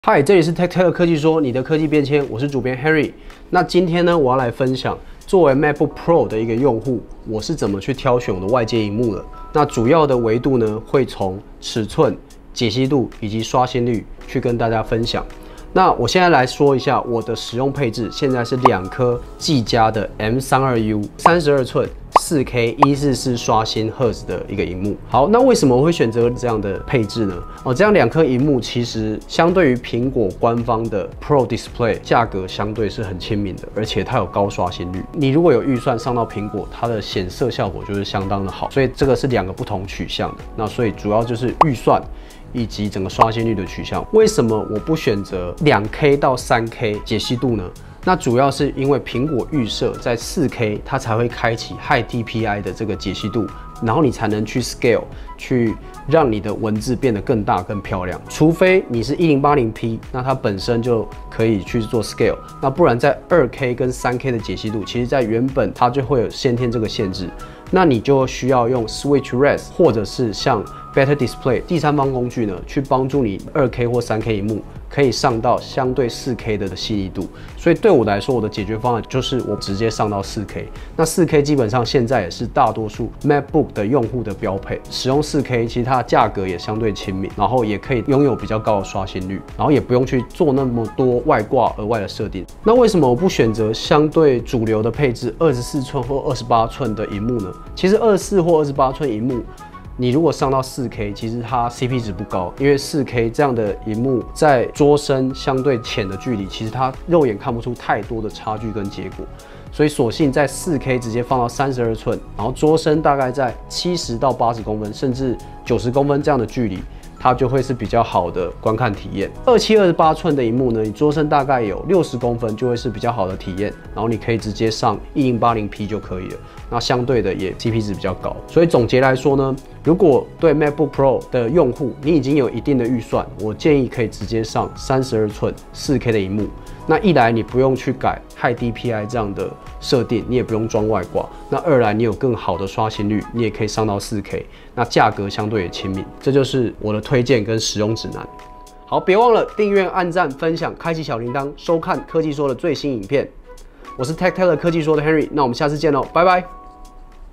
【科技說】MacBook 外接"雙"螢幕的最佳選擇!?｜4K、144Hz、60Hz｜Mac知識｜三分鐘說科技EP11
MacBook Pro外接螢幕挑選指南！

【熱門科技說影片盤點】

【開箱評測】迄今為止soundcore最強降噪真無線藍牙耳機｜Liberty 4 NC｜首款支援Google Fast Pair

 ‼️ 影片未經授權，禁止轉載 ‼️